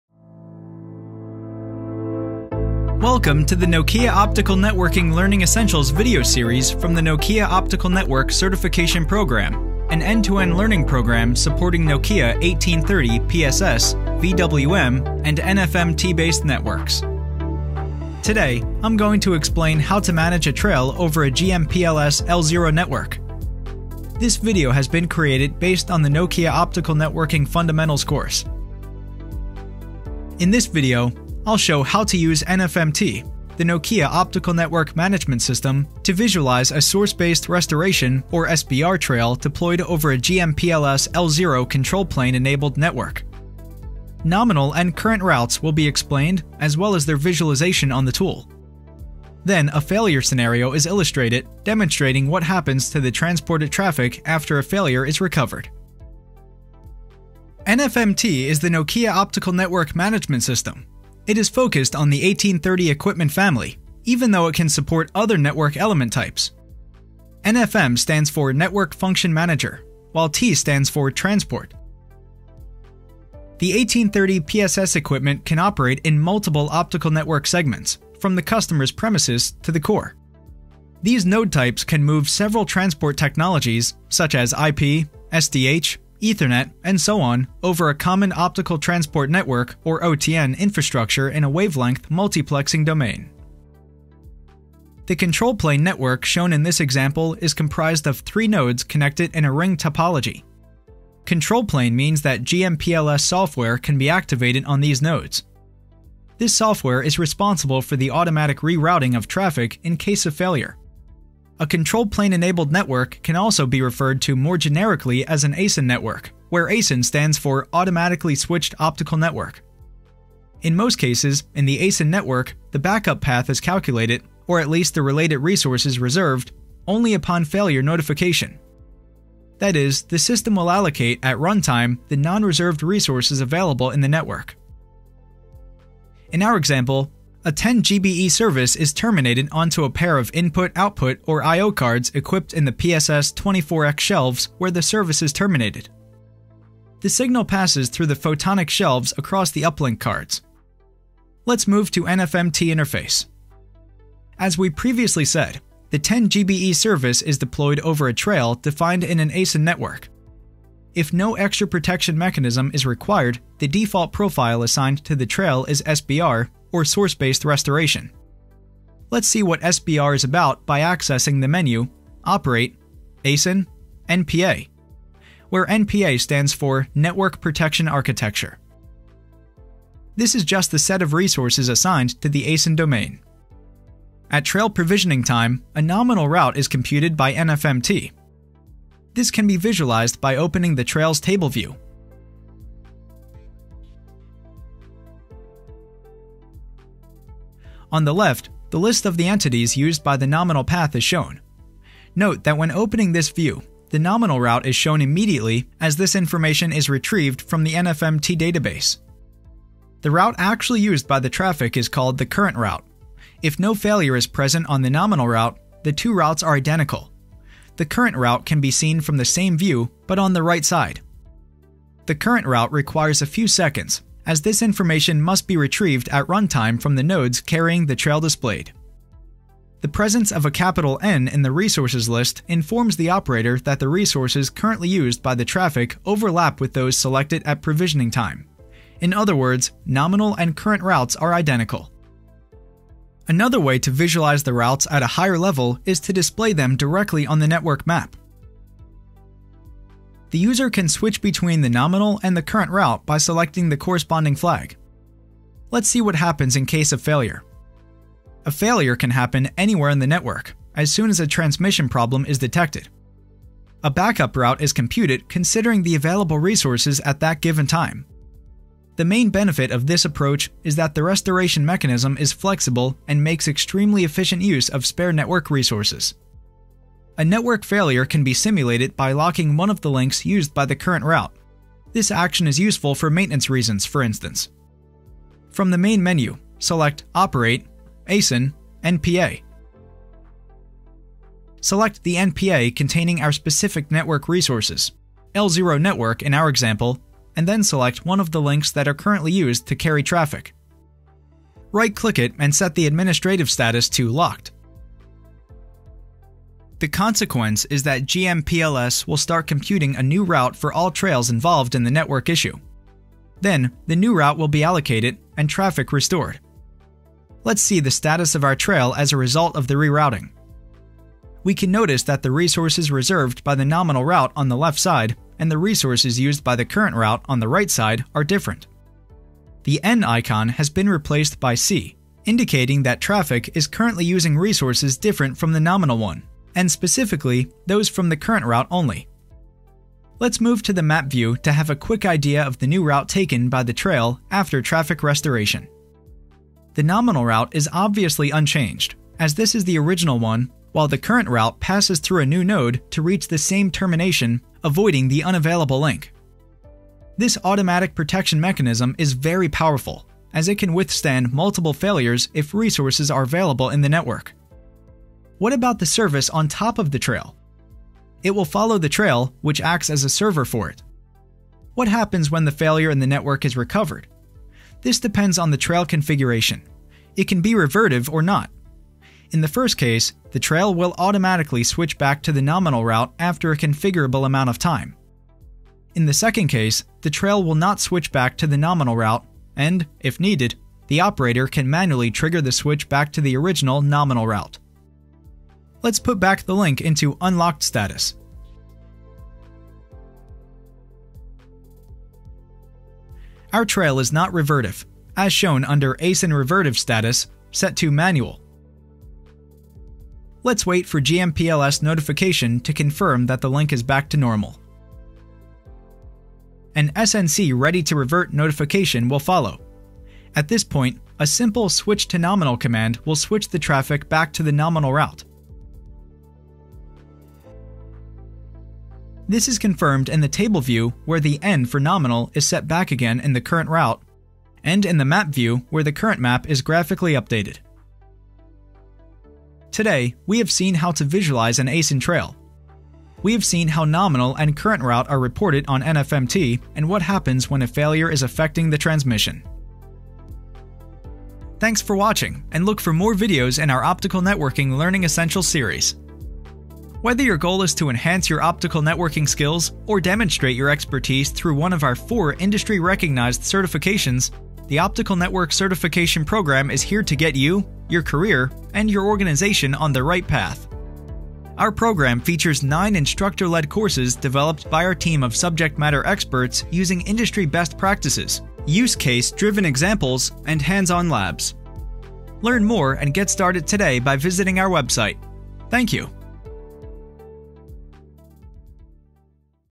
Manage a trail over a GMPLS L0 network
In this video, we explain how to manage a trail over a GMPLS L0 network. We'll show how to use NFM-T, the Nokia optical network management system, to visualize a Source Based Restoration (SBR) trail deployed over a GMPLS L0 control-plane-enabled network. Nominal and current routes will be explained, as well as their visualization on the tool. Then, a failure scenario is illustrated, demonstrating what happens to the transported traffic also after the failure is recovered.

This video is based on content from the Nokia Optical Network Certification (ONC) Program, an end-to-end learning program supporting Nokia 1830 PSS, VWM and NFM-T based networks.